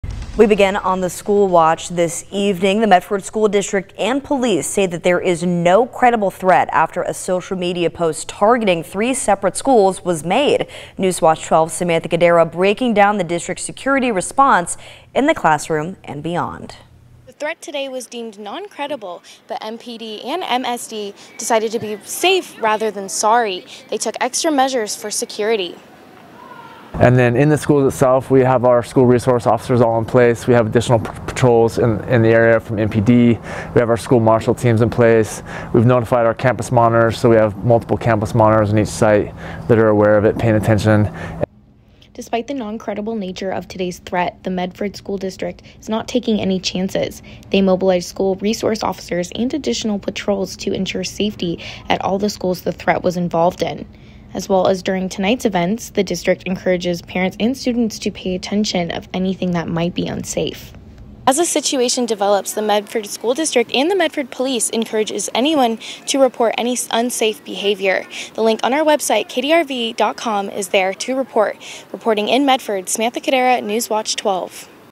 Medford School District ramping up security for schools, games after social media threat
NewsWatch 12's Samantha Kadera breaks down how the district works with Medford Police to keep students, staff and community members safe.